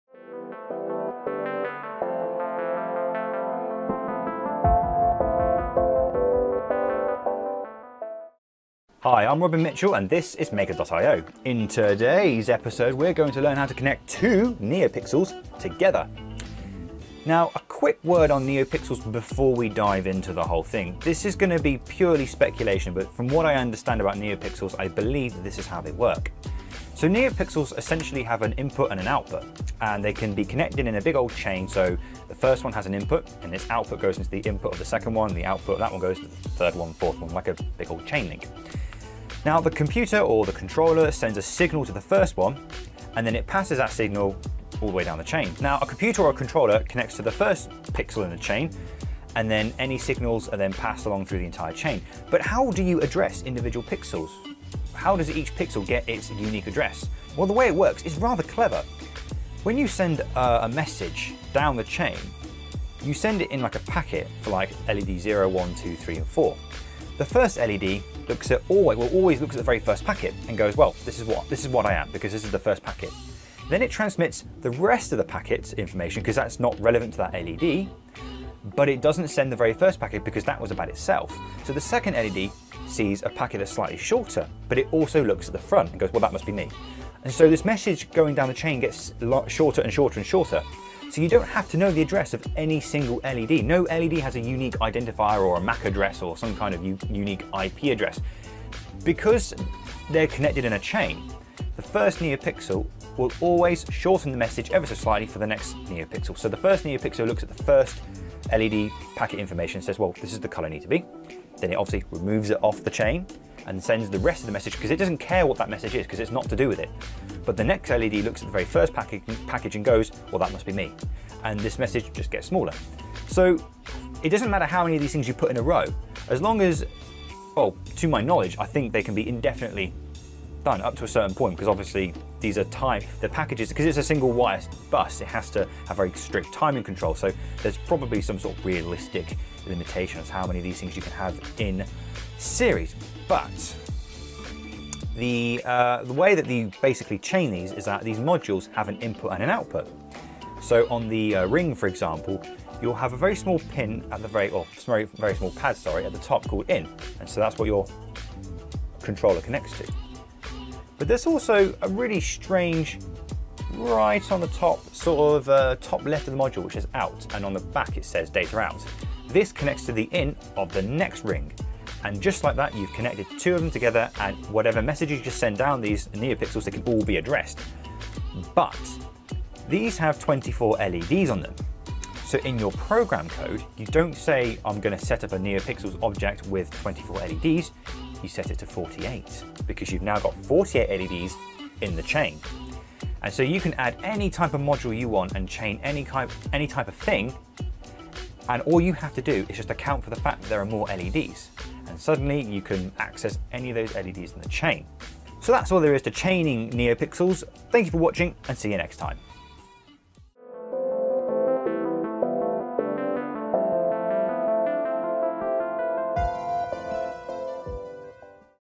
How to Set Up Multiple Neopixel Rings on a micro:bit – Maker.io Tutorial | Digi-Key Electronics
Using a micro:bit, Robin shows how to use multiple Neopixel rings.  Additionally, he explains fundamentally how Neopixels operate.